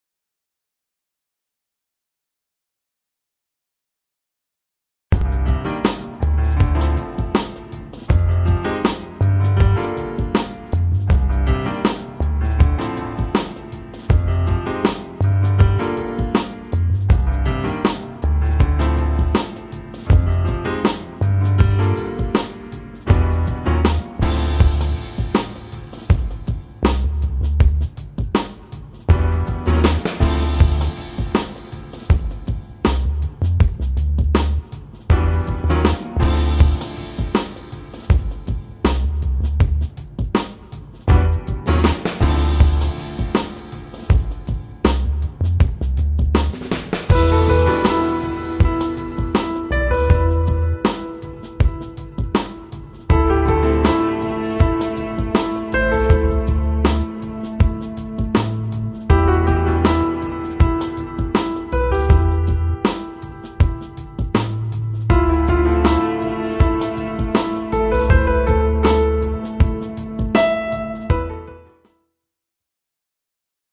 Peugeot 2008 occasion visible à Albertville présentée par Peugeot albertville
Cette Peugeot 2008 occasion disponible à Albertville dans le 73 est mise en vente par Peugeot albertville - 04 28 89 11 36 - tous les détails sont